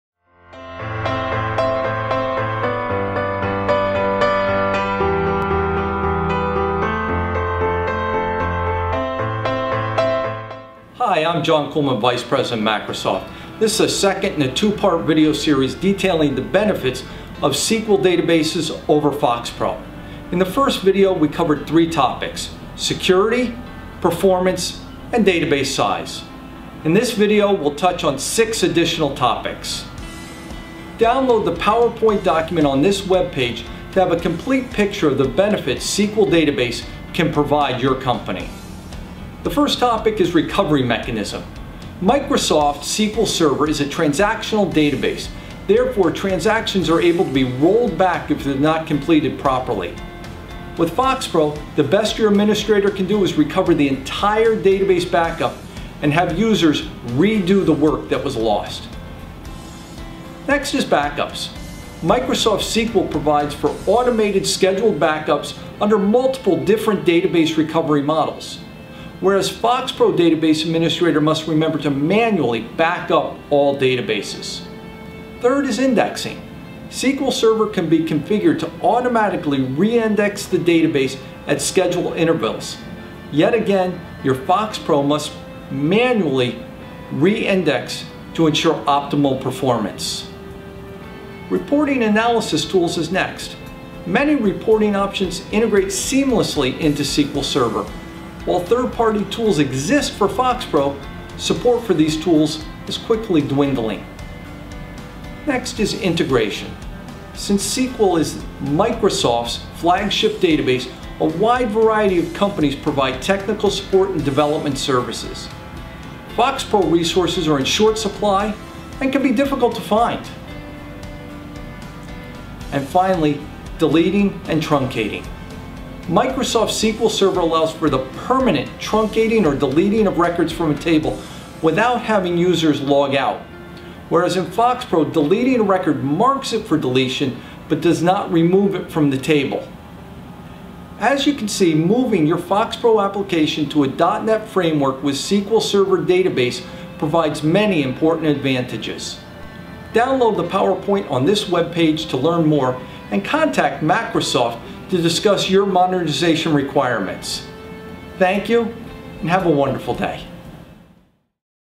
10 Benefits You Need To Know When Converting VFP to SQL Database Part 2
This is the second in a two-part video series detailing the benefits of SQL databases over FoxPro. In the first video, we covered three topics security, performance, and database size. In this video, we'll touch on six additional topics. Download the PowerPoint document on this webpage to have a complete picture of the benefits SQL database can provide your company.

The first topic is the recovery mechanism. Microsoft SQL server is a transactional database. Therefore, transactions are able to be rolled back if it is not completed properly. With FoxPro, the best your administrator can do is recover the entire database backup and have users redo the work that was lost.

Next is Backup. Microsoft SQL provides for automated scheduled backups under multiple different database recovery models, whereas FoxPro database administrators must remember to manually back up all databases.

The third is Indexing. SQL Server can be configured to automatically reindex the database at scheduled intervals. Yet again, your Fox Pro must manually reindex to ensure optimal performance. Reporting analysis tools are next. Many reporting options integrate seamlessly into SQL Server. While third-party tools exist for FoxPro, support for these tools is quickly dwindling.

Next is Integration. Since SQL is Microsoft's flagship database, a wide variety of companies provide technical support and development services. Fox pro resources are in short supply and can be difficult to find. And finally, deleting and truncating.

Microsoft SQL Server allows for the permanent truncating or deleting of records from a table without having users log out. Whereas in FoxPro deleting a record marks it for deletion but does not remove it from the table. As you can see, moving your FoxPro application to a .net framework with an SQL Server database provides many important advantages.

Download the PowerPoint on this webpage to learn more and contact Microsoft to discuss your monetization requirements. Thank you and have a wonderful day.